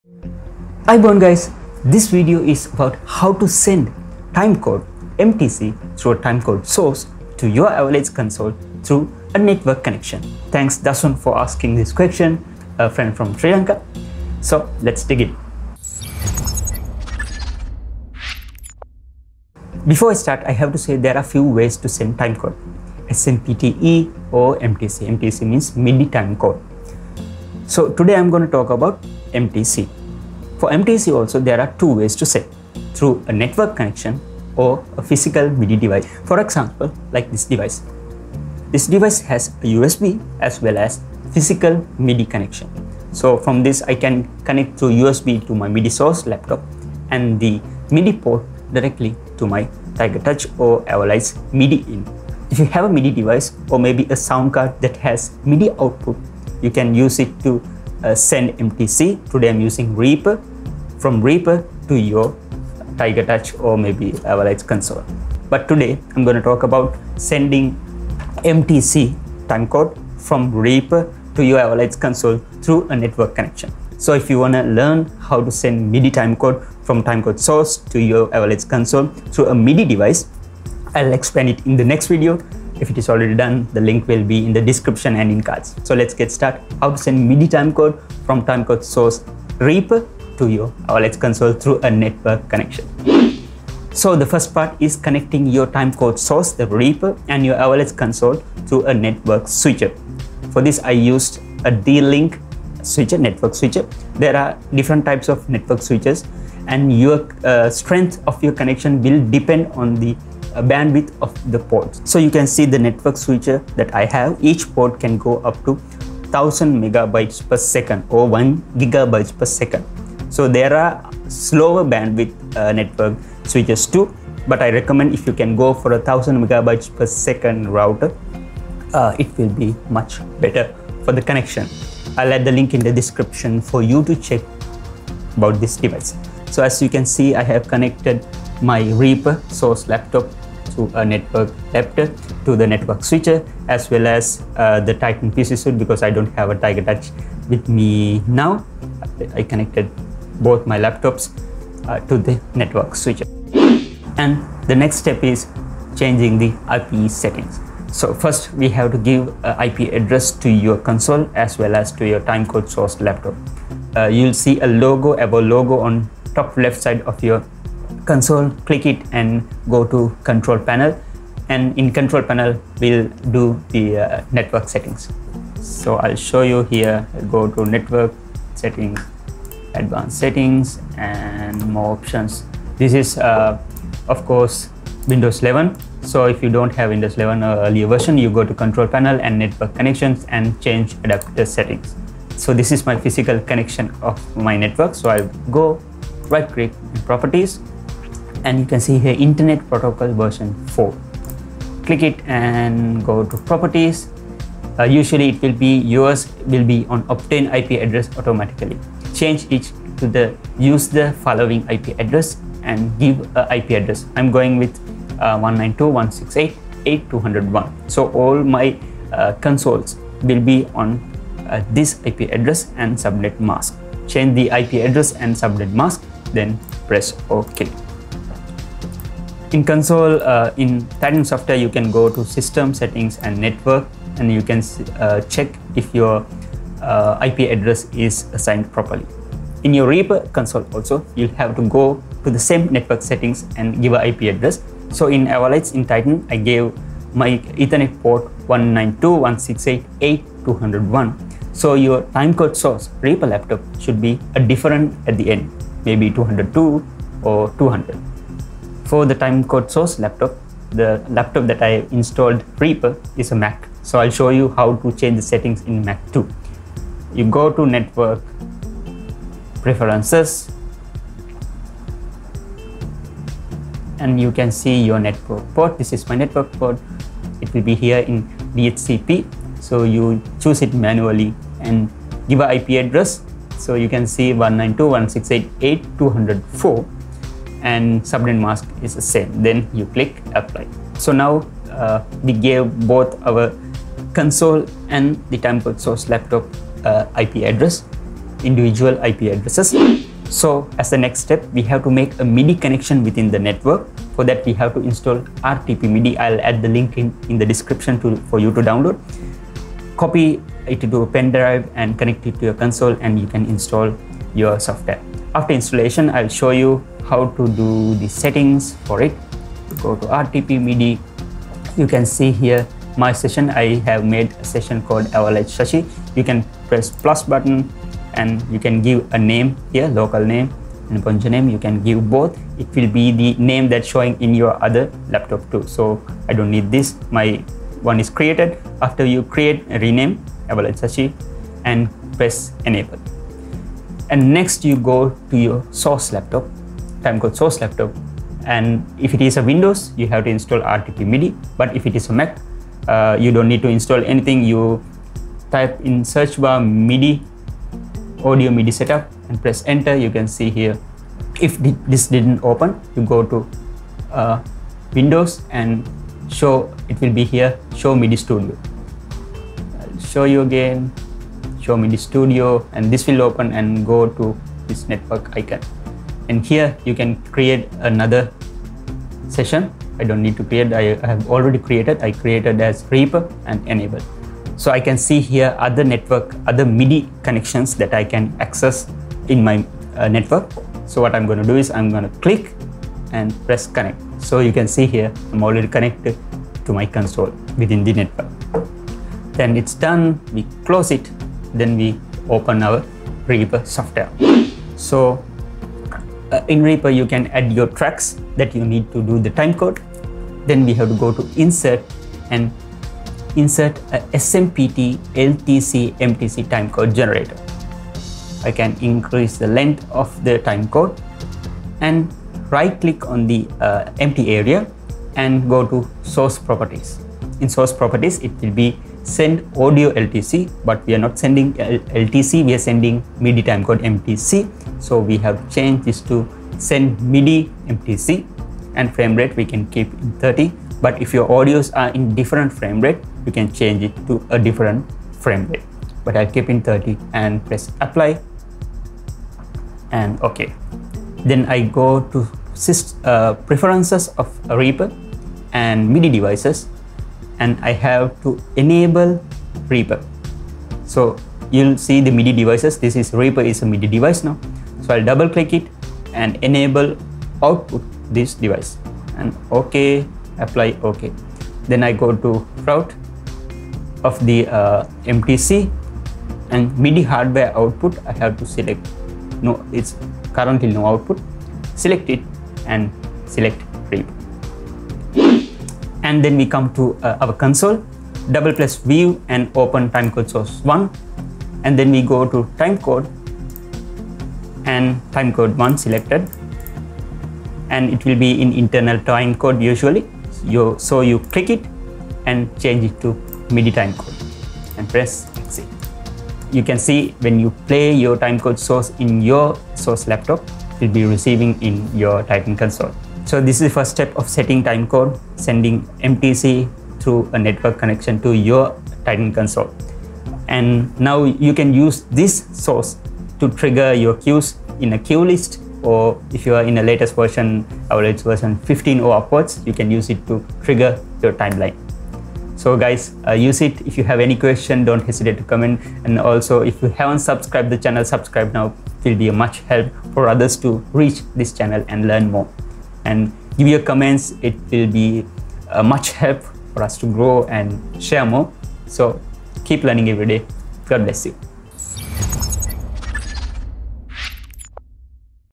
How to send MIDI Timecode through a Network to your Avolites console
One way to send timecode from Reaper to your Avolites console.
Keep in touch for more ways to send timecode from reaper to Avolites consoles through midi devices as well as LTC or SMPTE.

Chapters:
0:00 Intro
0:20 Important message
1:27 Network Connection Between Reaper and Console
2:34 Giving IP Addresses to Laptop and Console
5:00 Making MIDI Connection between 2 devices within network
5:21 Changing Settings in RTP Midi
6:44 Setting up Reaper
8.05 Setting Avolites Titan
8.33 Conclusion

Keep in touch and keep learning every day